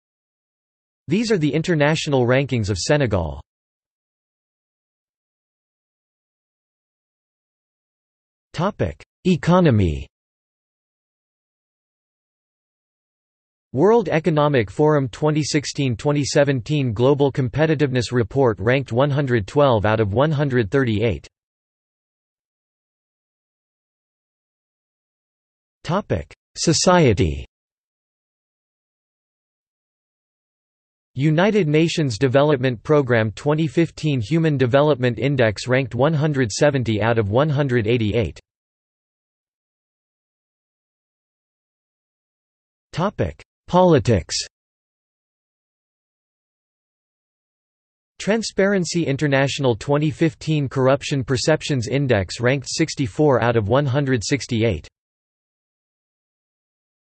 International rankings of Senegal | Wikipedia audio article
00:00:04 1 Economy
00:00:22 2 Society
00:00:39 3 Politics

- Socrates

SUMMARY
These are the international rankings of  Senegal.